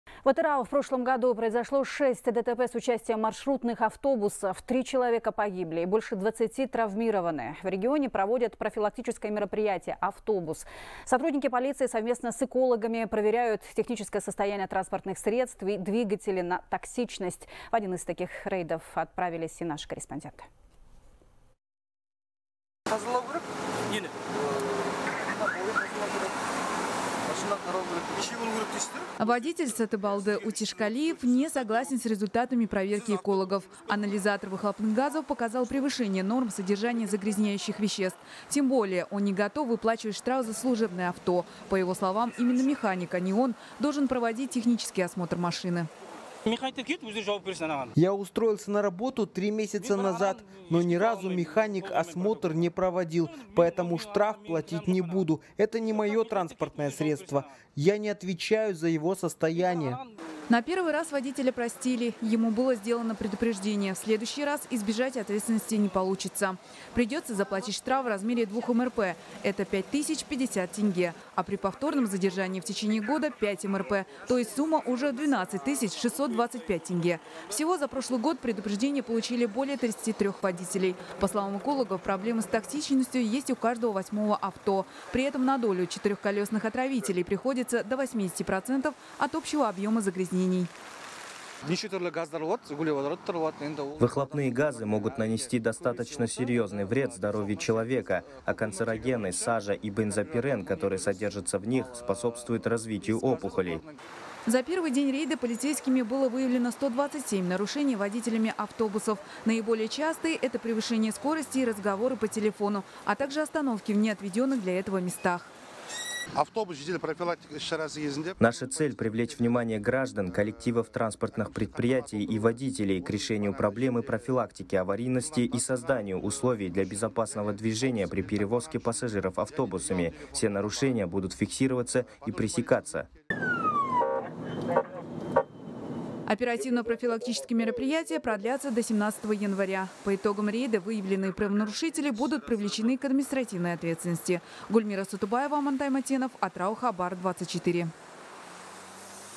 Профилактическое мероприятие «Автобус» проводят в Атырау
В Атырау в прошлом году произошло 6 ДТП с участием маршрутных автобусов. Три человека погибли и больше 20-ти травмированы, сообщает корреспондент «Хабар 24».

В регионе проводят профилактическое мероприятие «Автобус». Сотрудники полиции совместно с экологами проверяют техническое состояние транспортных средств, двигатели на токсичность. В один из таких рейдов отправились и наши корреспонденты.